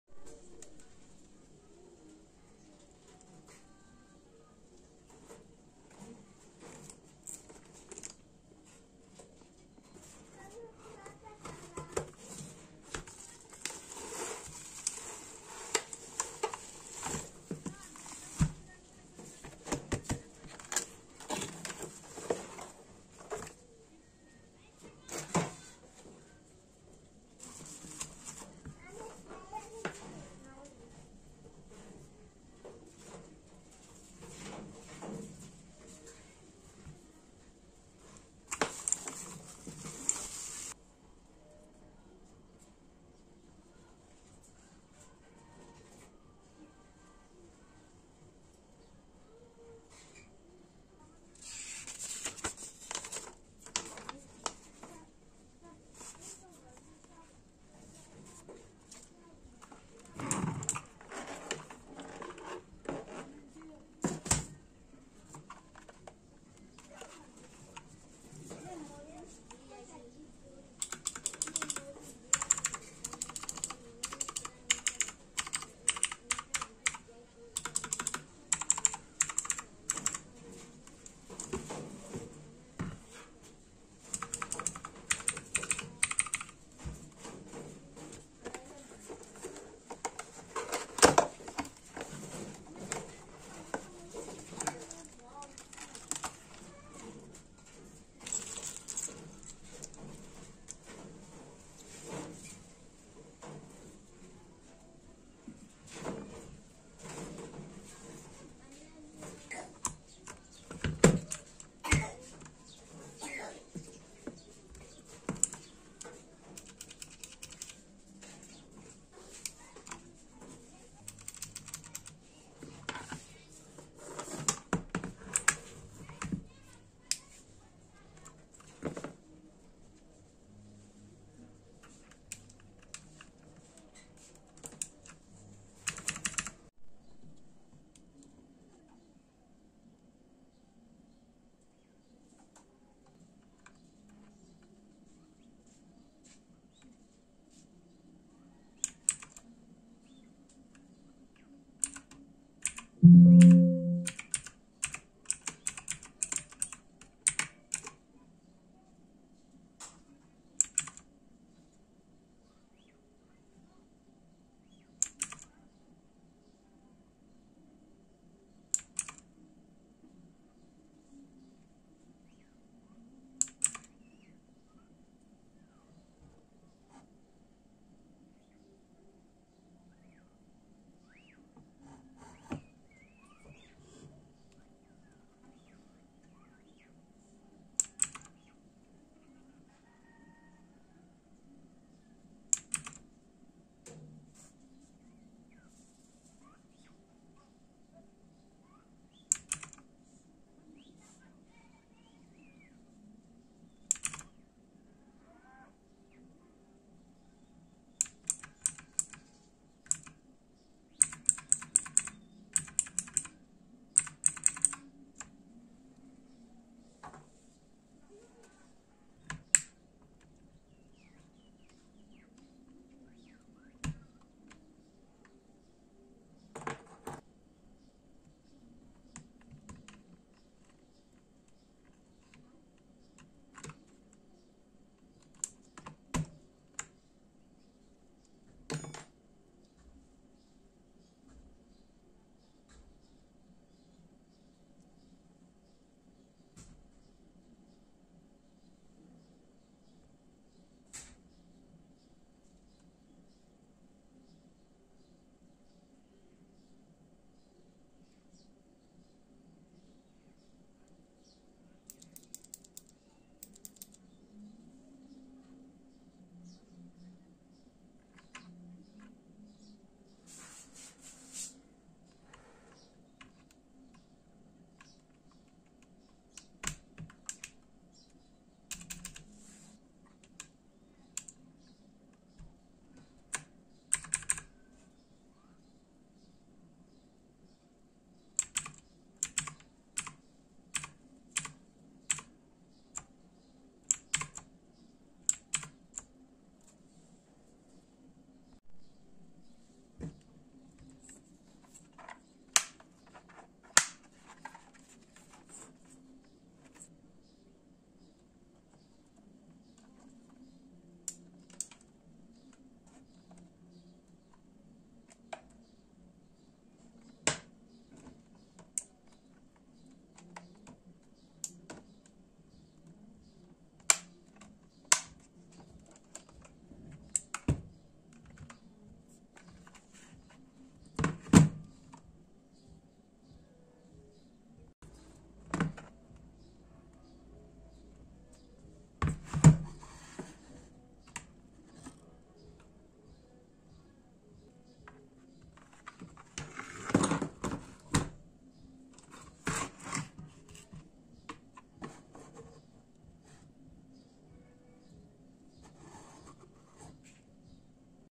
T-DAGGER NAXOS T-TGK310 Gaming Mechanical Keyboard
Unboxing of T-DAGGER T-TGK310 Gaming Mechanical Keyboard.
00:05 - Unboxing
00:33 - Usb Cable lenght - 150 cm
00:43 - User Manual
01:10 - Key test
01:30 - Keycap and switcher remover
02:20 - lighting modes
03:47 - Removing Switches
04:02 - Outemu Blue Switches